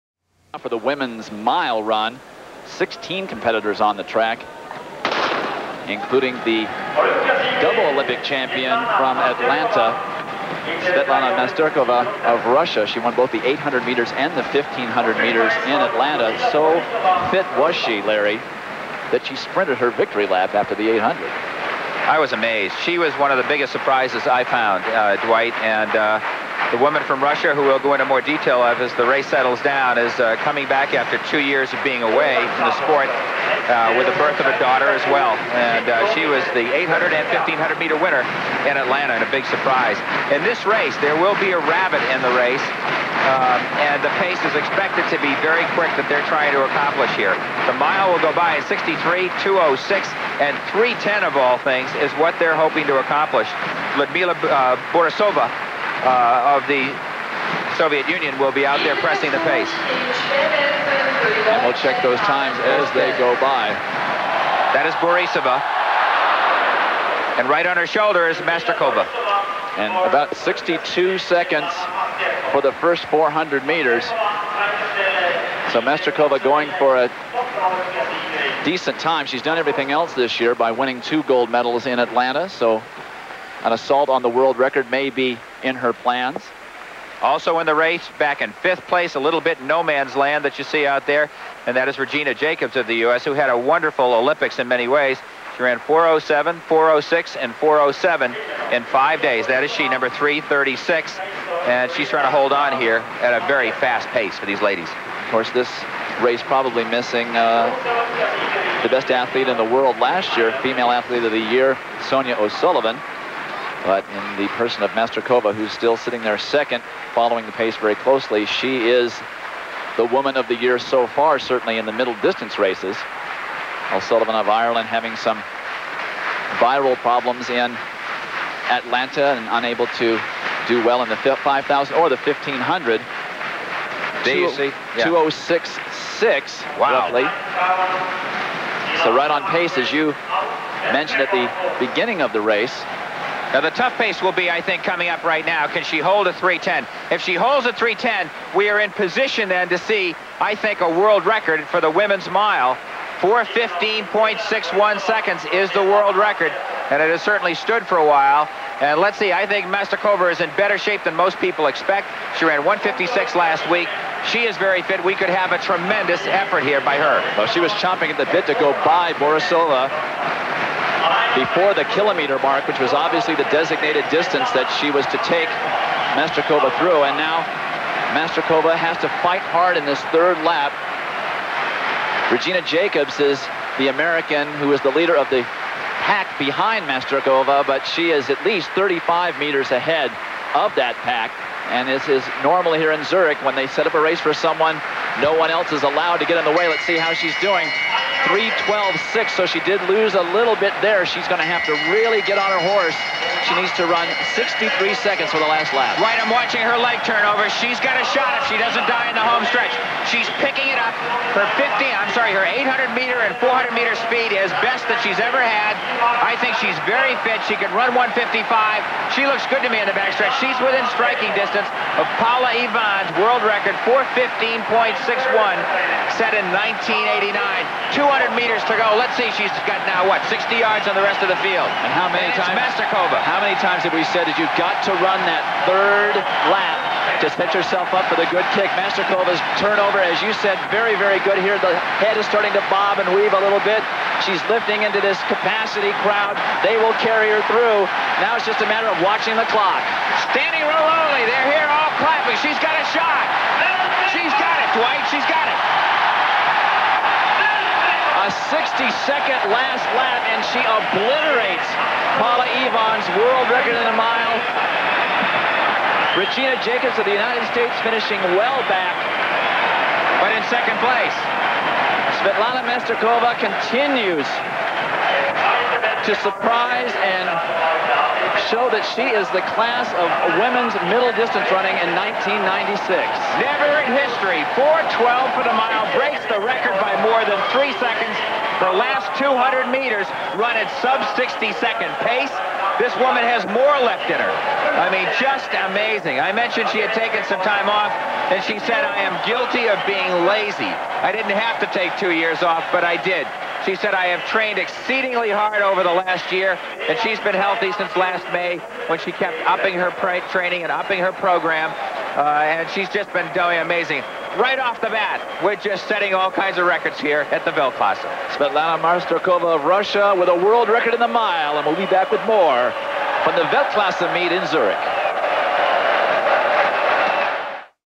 Svetlana Masterkova - Women's Mile (WR) - 1996 Zürich Weltklasse
Weltklasse Golden Four
Zürich, Switzerland
August 14, 1996
Women’s Mile Results:
     1) Svetlana Masterkova – 4:12.56
     2) Regina Jacobs – 4:22.20
     3) Anna Brzezinska – 4:22.96
     4) Leah Pells – 4:23.28
     5) Carla Sacramento – 4:23.67
     6) Theresia Kiesl – 4:24.37
     7) Paula Radcliffe – 4:24.94
     8) Yekaterina Podkopayeva – 4:26.96
     9) Margaret Crowley – 4:26.99
    10) Malgorzata Rydz – 4:27.37
    11) Frederique Quentin – 4:27.43
    12) Sinead Delahunty – 4:28.11
    13) Sarah Thorsewt – 4:28.91
    14) Margarita Maruseva – 4:30.48
    15) Klandine Britzner-Ducret – 4:31.17